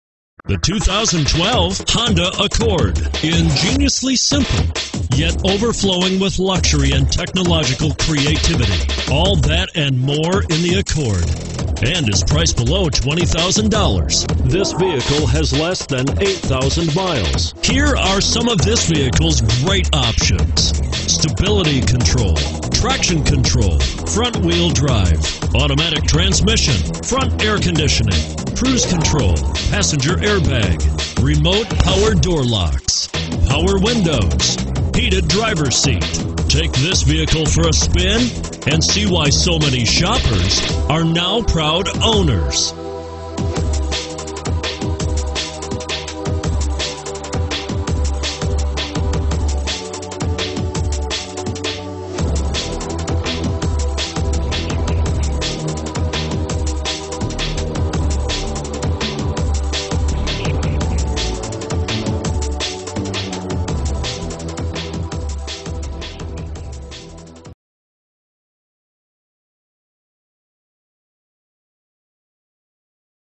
2012 Honda Accord DC, Maryland, and Virginia 91248
2012 Honda Accord 2.4 SE - $18595

Don't miss this 2012 Honda Accord. It's equipped with Automatic transmission and features a Crystal Black Pearl exterior. With 7877 miles, You'll want to take this  home. Make a great choice today.